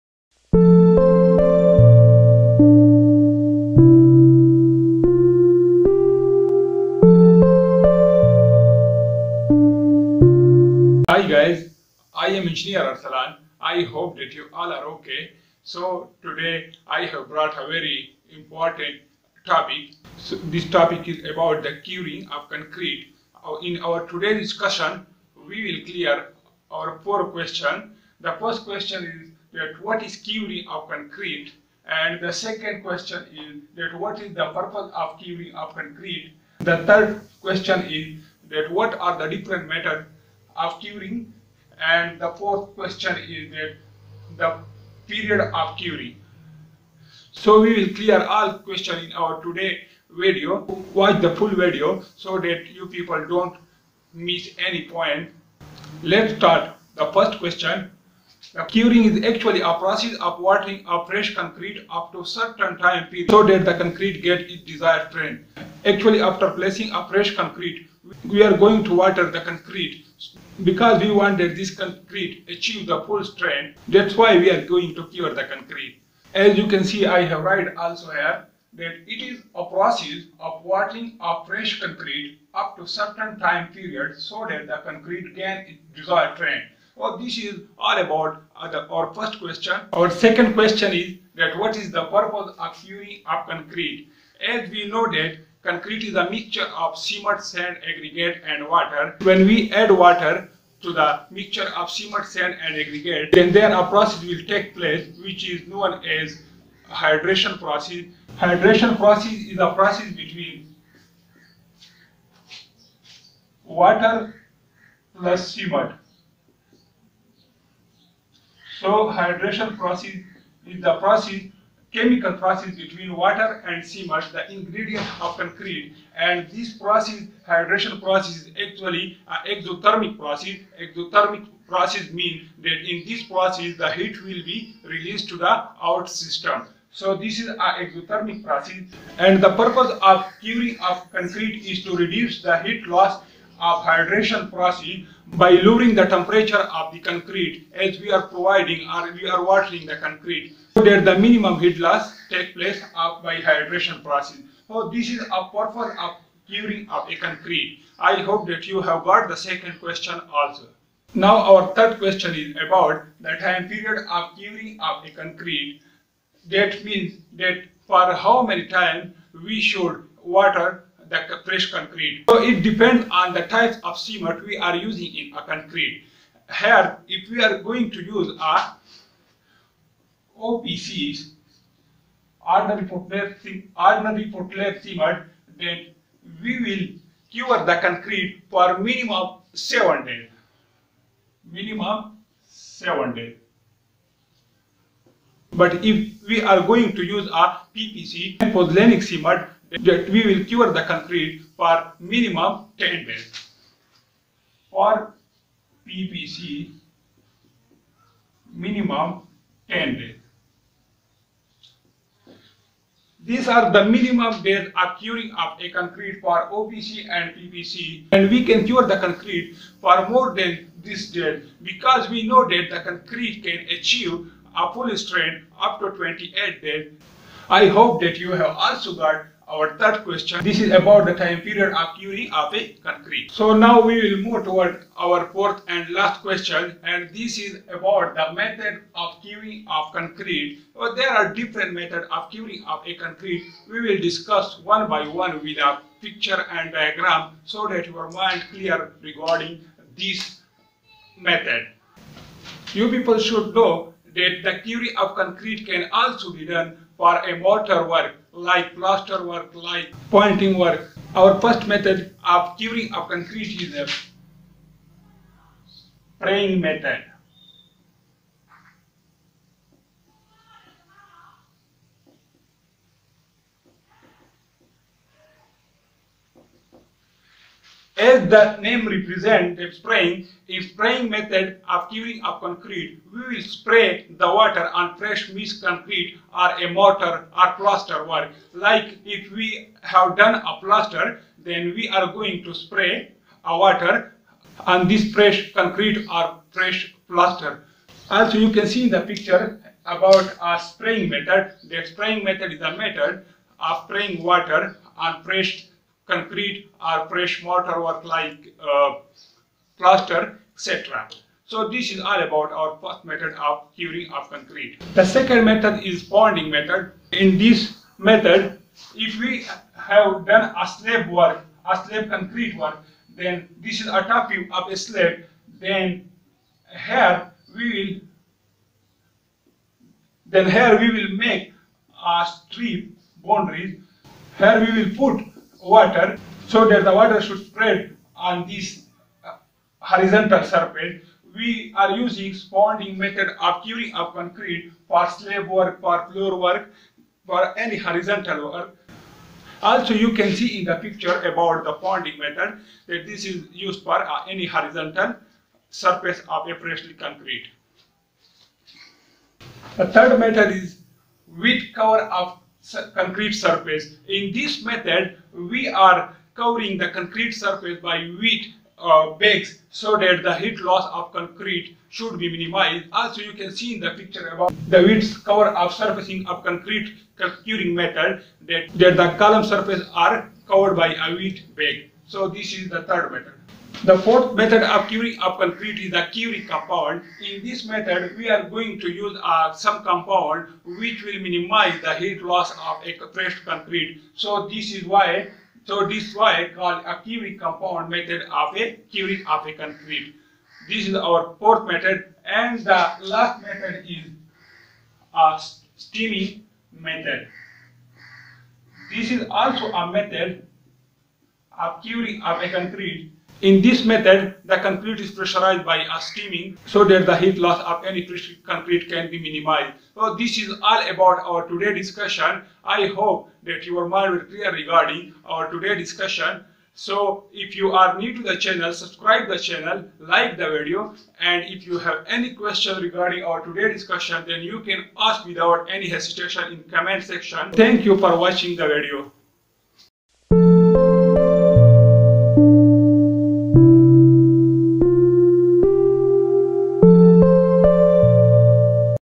Curing of Concrete ? | Methods of Curing ? | Purpose of Curing ?
What is Curing of Concrete ?
What is Purpose of Curing ?
What are Methods of Curing ?
What is Time Period of Curing ?